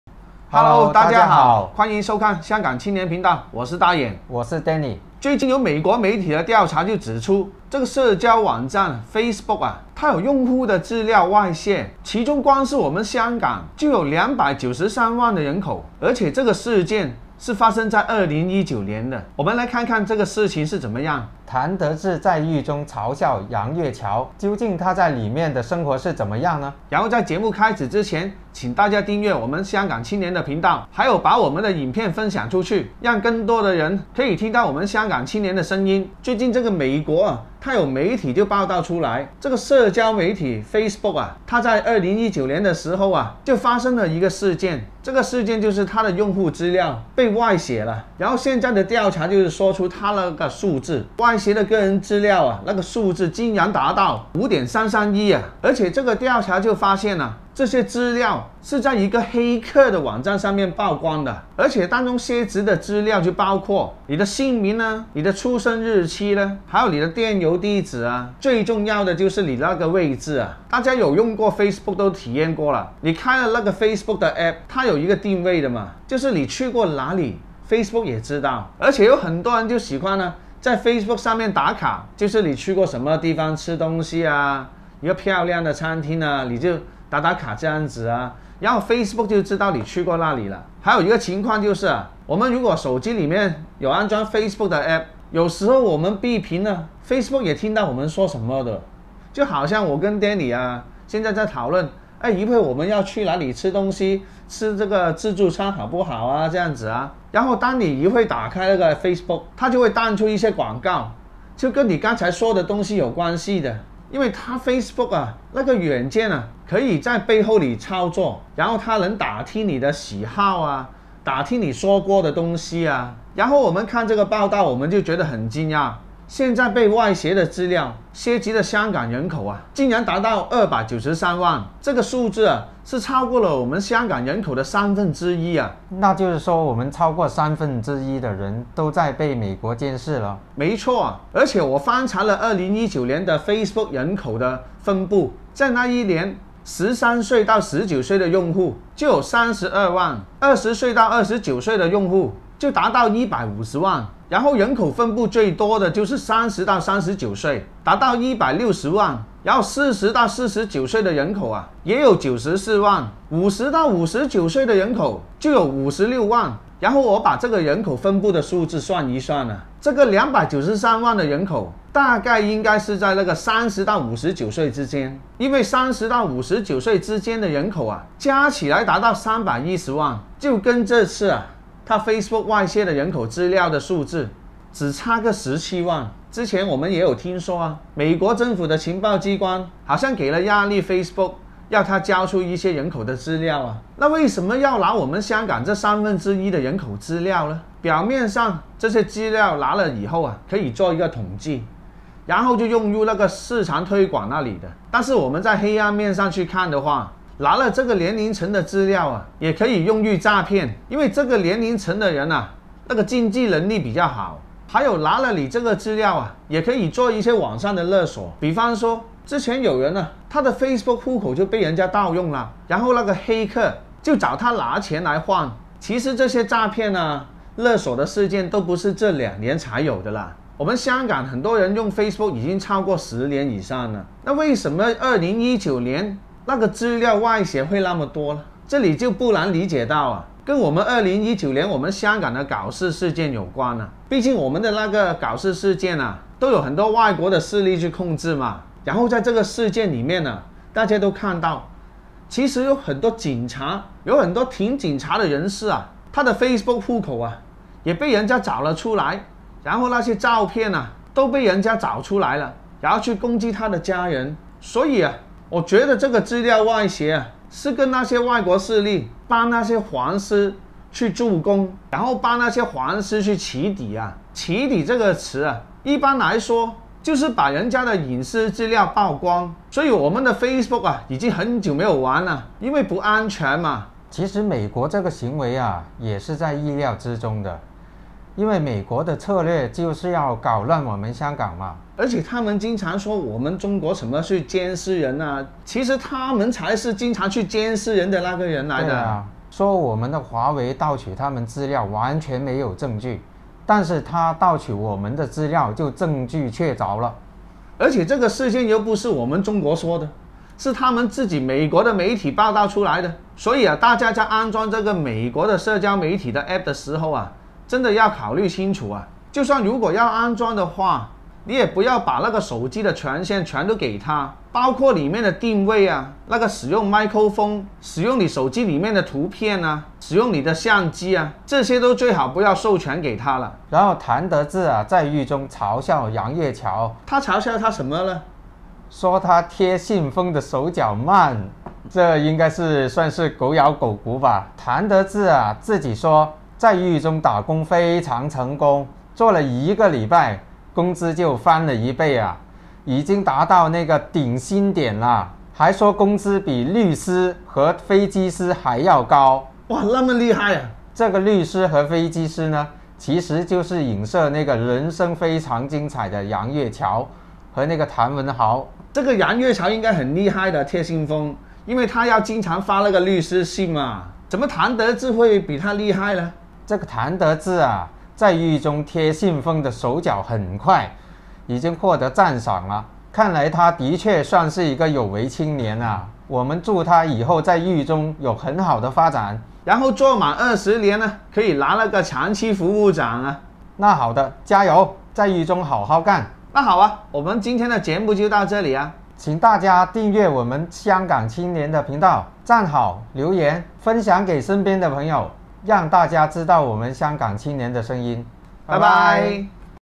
FB外洩港人資料 助攻黃絲起底 譚得志 嘲笑 楊岳橋潦倒 / 大眼 DANNY ［國語］
FB外洩293萬港人資料 助攻黃絲起底  譚得志 嘲笑 楊岳橋潦倒 / 大眼 DANNY ［國語］